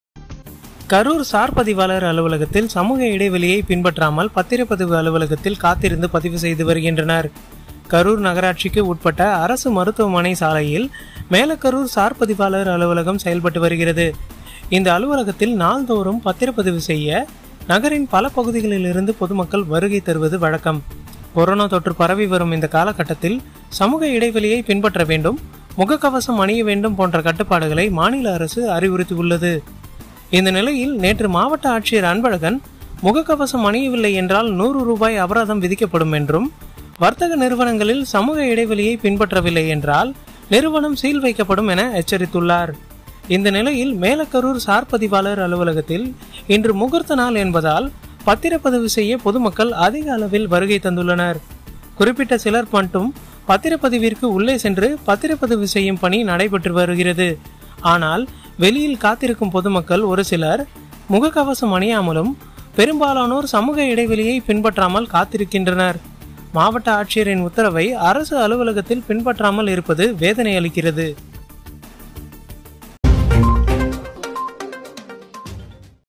கரூர் சார் பதிவாளர் அலுவலகத்தில் சமூக இடைவெளியை பின்பற்றாமல் பத்திர பதிவு அலுவலகத்தில் காத்திருந்து
கரூர் சார் பதிவாளர் அலுவலகத்தில் சமூக இடைவெளியை பின்பற்றாமல் பத்திர பதிவு அலுவலகத்தில் காத்திருந்து பதிவு செய்து வருகின்றனர்.